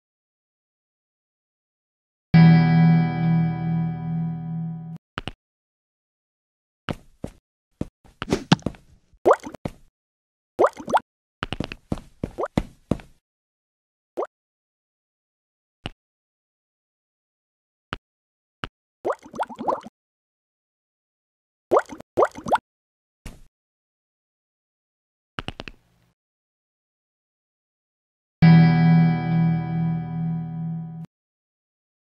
No title 4 (2016) - 청강 애니메이션 2016 1학년 2학기 과제물
1학년 '3D애니메이션제작'수업 결과물입니다
Animation dept., Chungkang Cultural Industries
This work is made from '3D Animation Project' Freshman